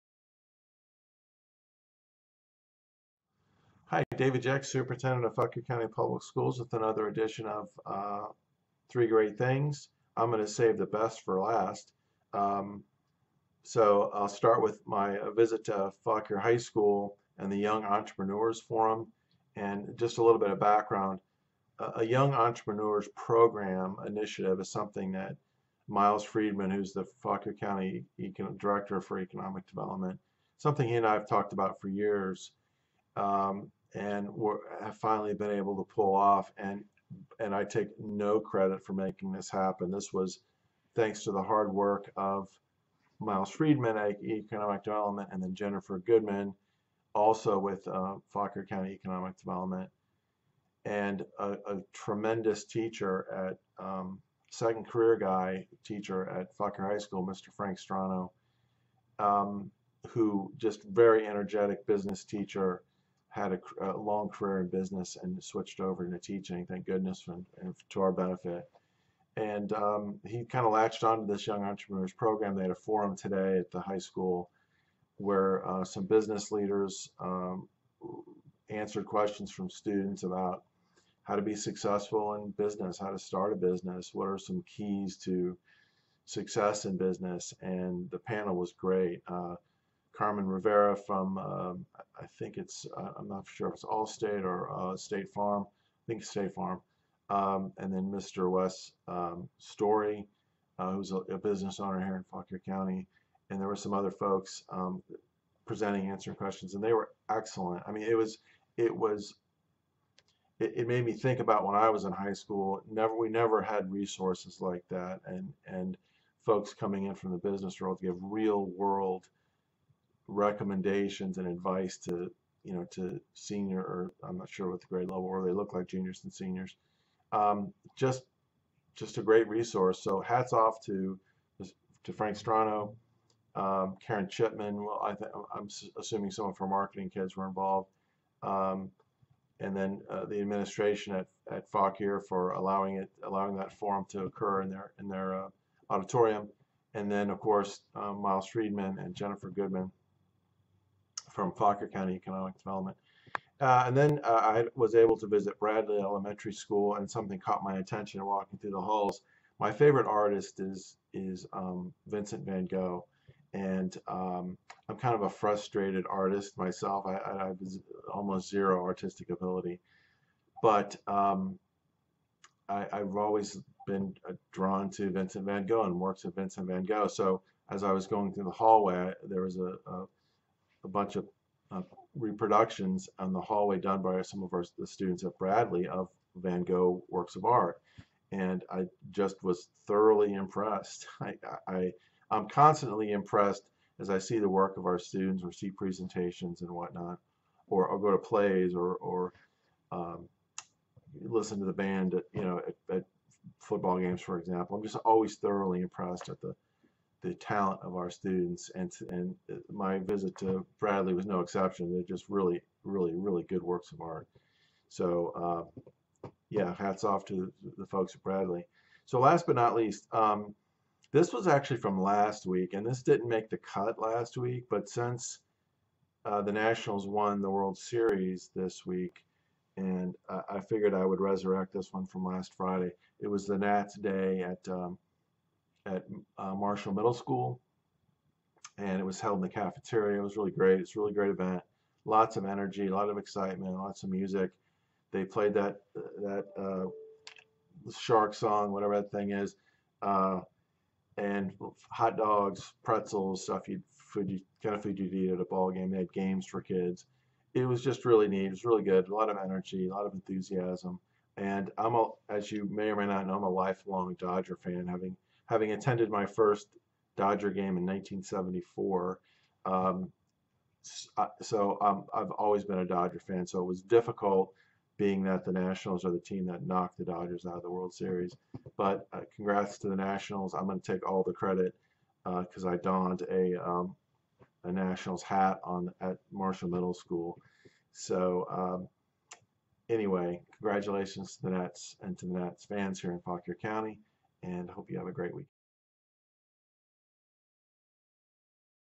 Three Great Things 11 1 19
Dr. Jeck shares three great things he saw last week.